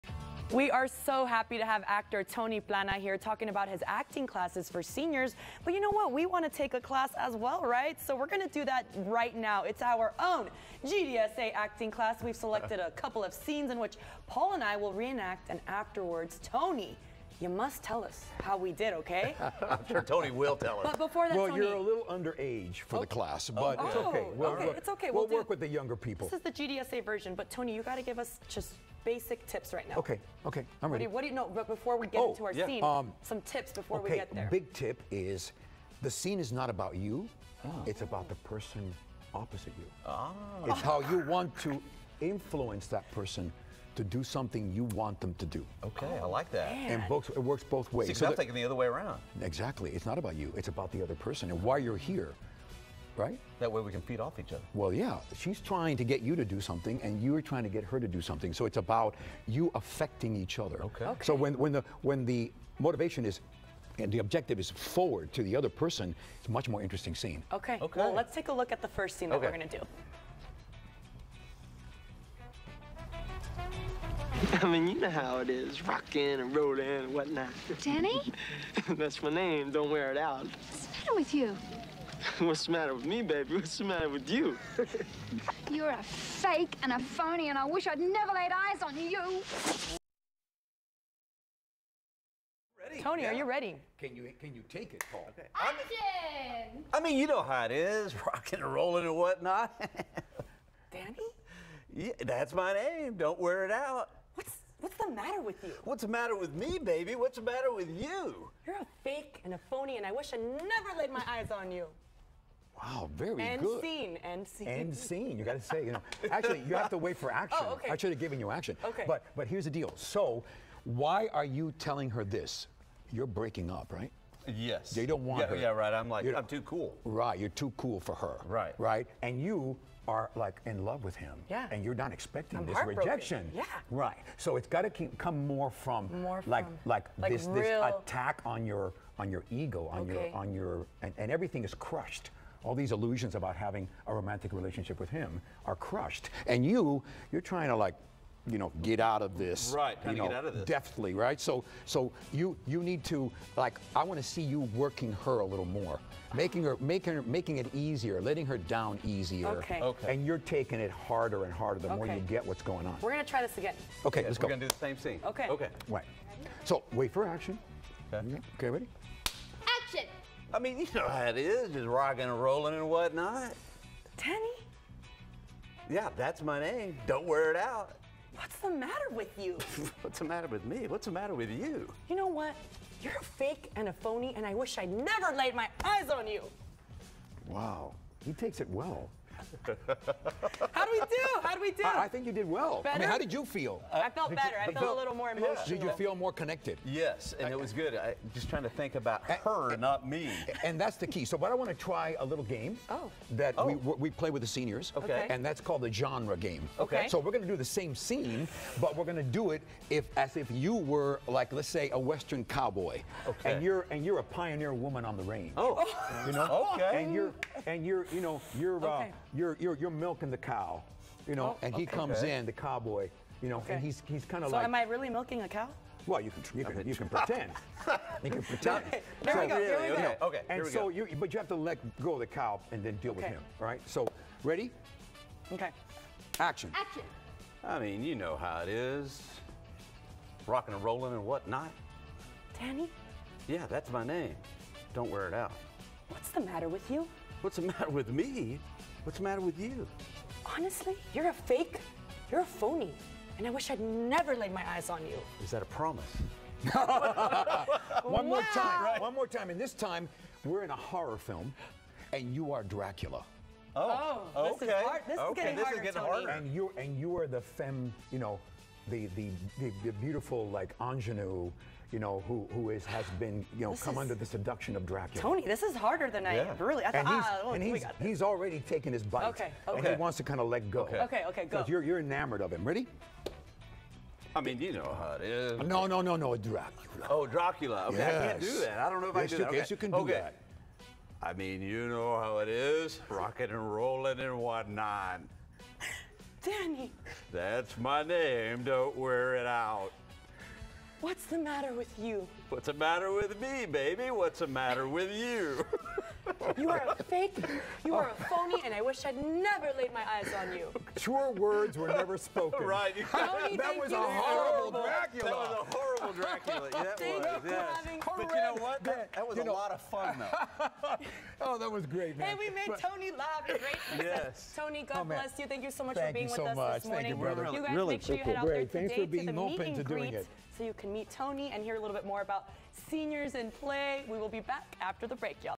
Tony Plana teaches the first ever GDSA acting class | Great Day SA
Roma and Paul get some acting lessons from Tony Plana after recreating an iconic movie scene